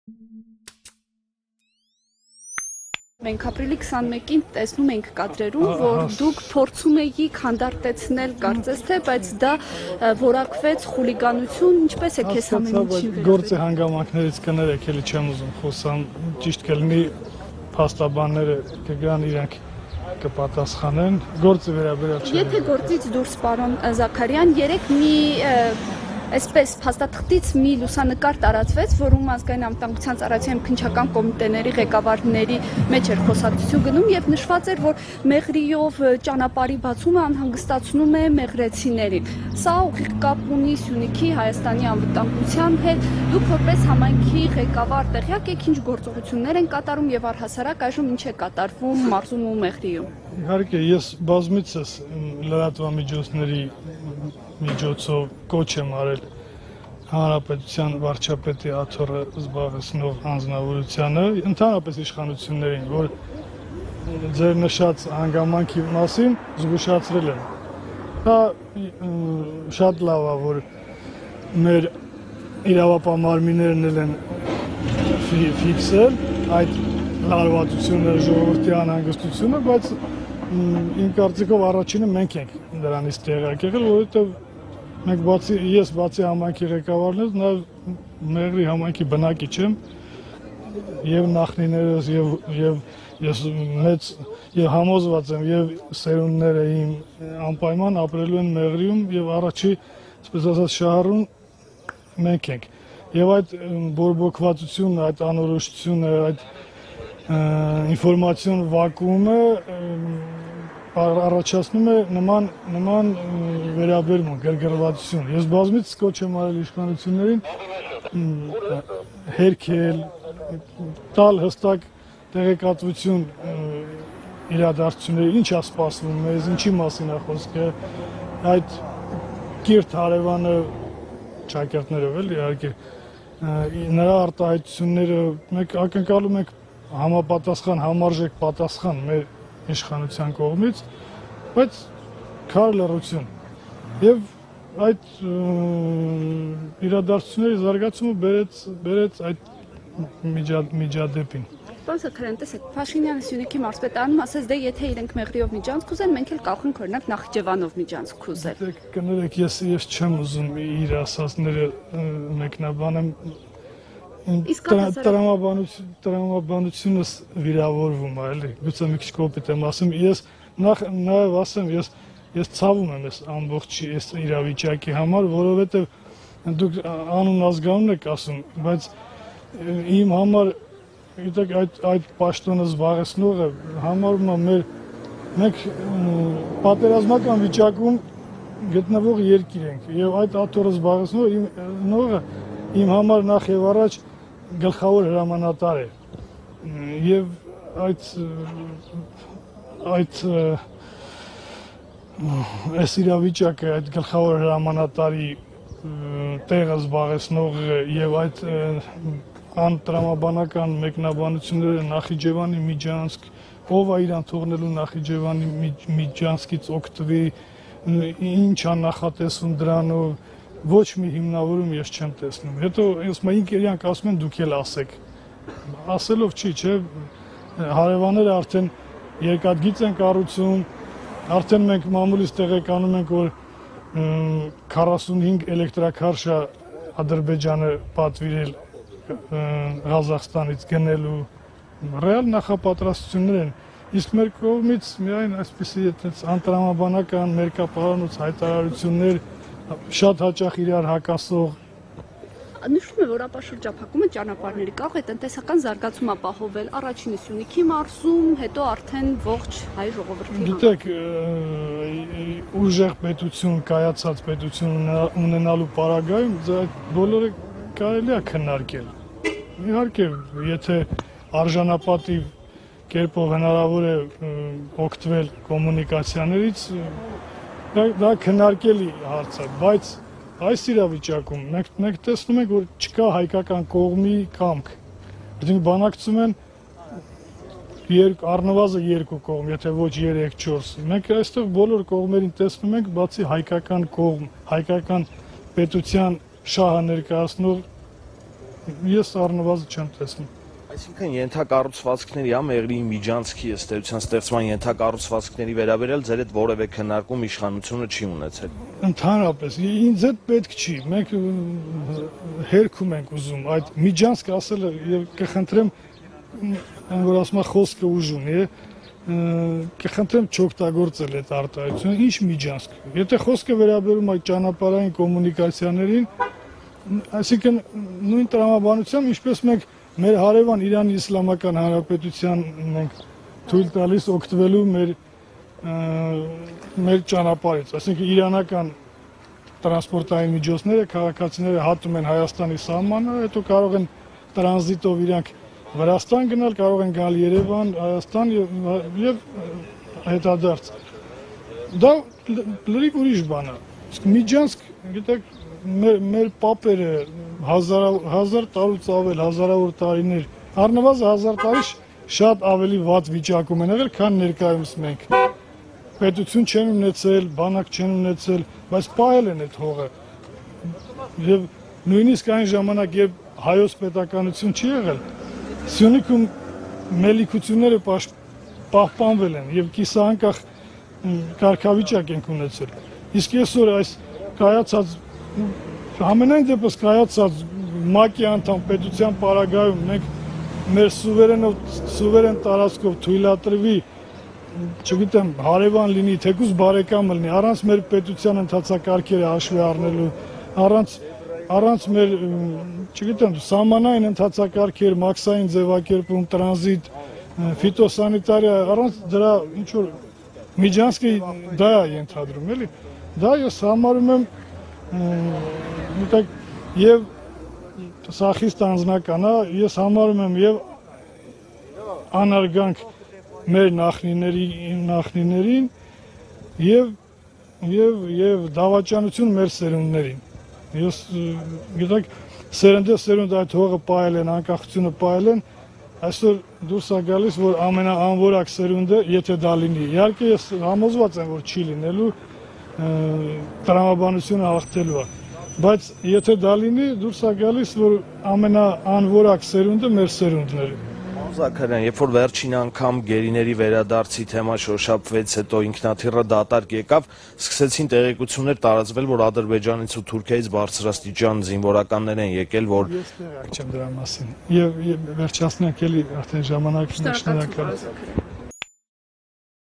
Տրամաբանությունս վիրավորվում է Փաշինյանին մեկնաբանելիս. Մեղրիի քաղաքապետ
Ադրբեջանը էլեկտրաքարշեր է պատվիրում, իսկ մեր մոտ միայն անտրամաբանական ելույթներ են. Մխիթար Զաքարյան